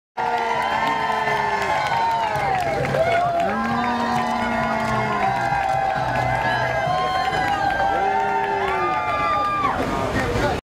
Condola Rashad and Orlando Bloom at the 'Good Morning Ame...
Condola Rashad and Orlando Bloom at the 'Good Morning America' studio in New York, NY, on 8/22/13.